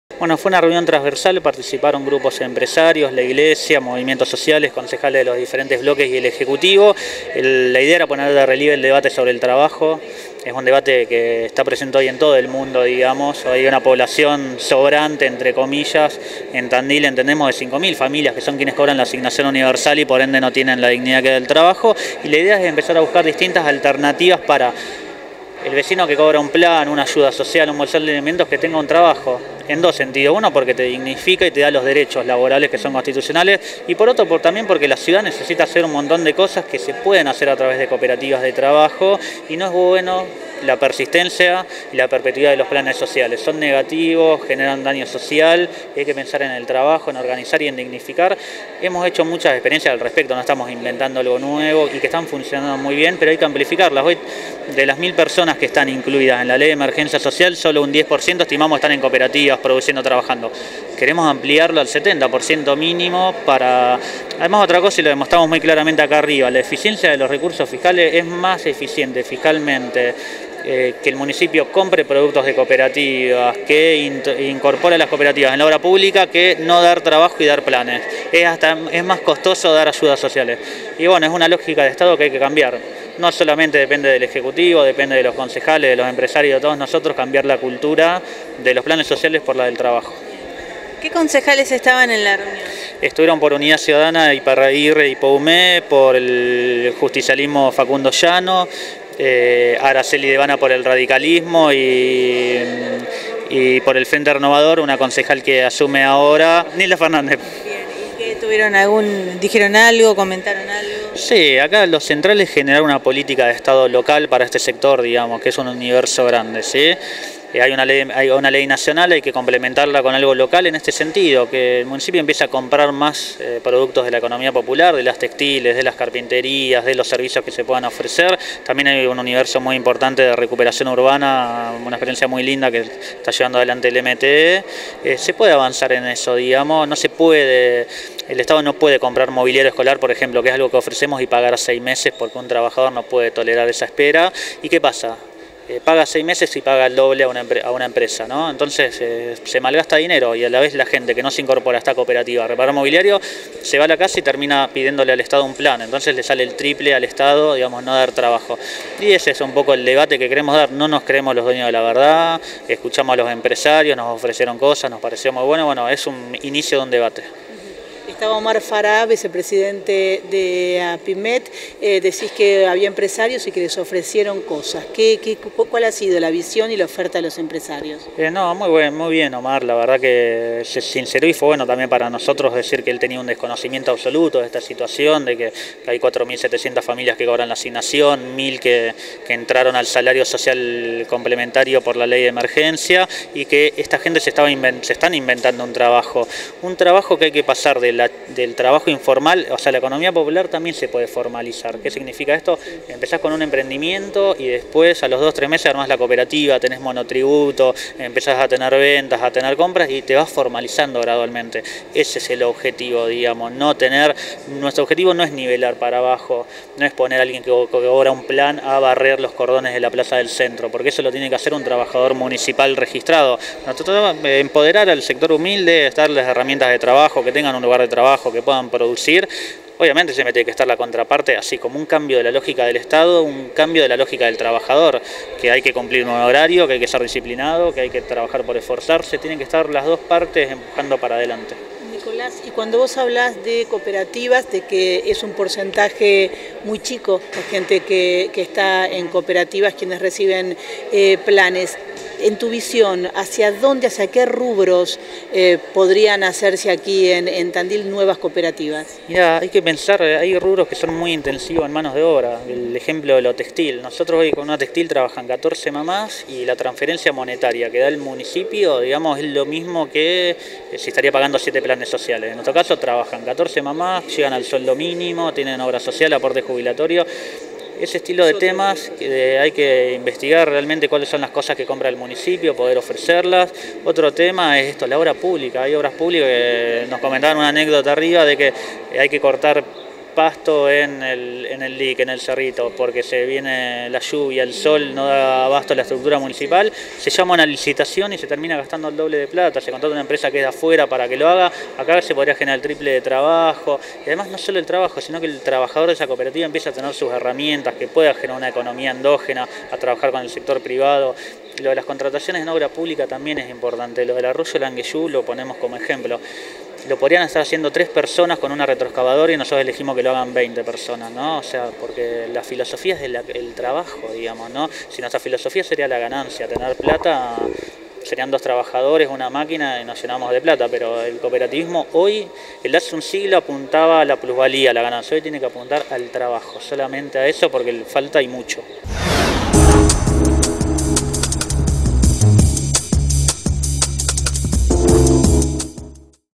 DEBATE POR TRABAJO EN TANDIL
Se reunieron en el Palacio Municipal diferentes organizaciones para debatir sobre el trabajo en nuestra ciudad. Dialogamos sobre esto con Nicolás Carrillo, referente  del Movimiento Evita.